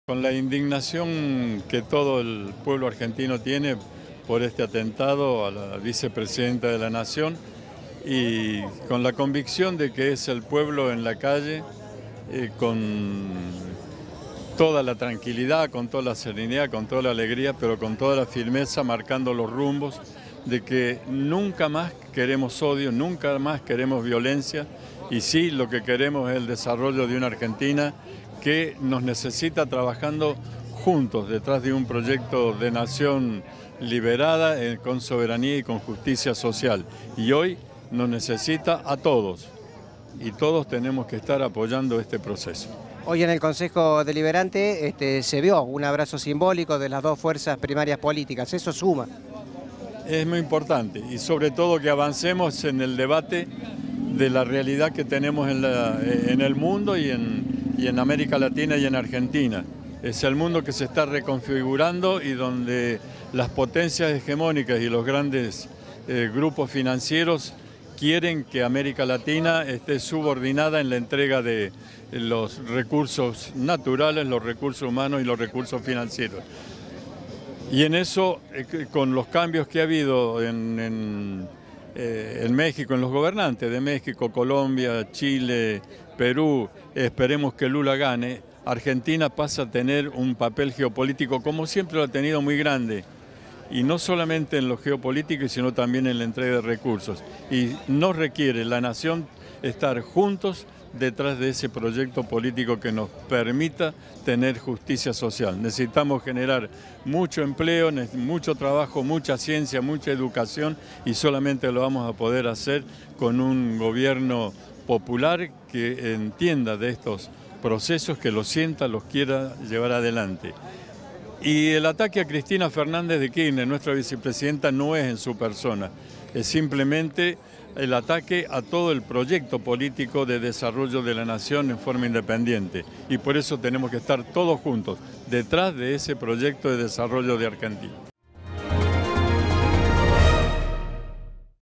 MARCHA EN DEFENSA DE LA DEMOCRACIA Y EN SOLIDARIDAD CON LA VICEPRESIDENTA DE LA NACIÓN
Alberto Cantero - Ex rector UNRC - Ex intendente municipal